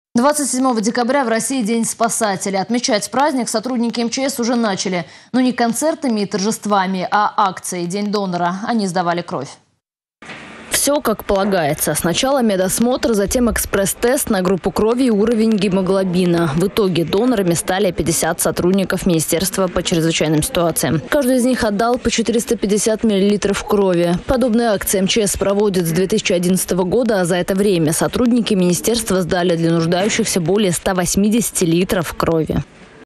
Сотрудники МЧС сдают кровь
27 декабря в России  - День спасателя. Отмечать праздник сотрудники МЧС уже начали. Но не концертами и торжествами, а акцией «День донора». Они сдавали кровь.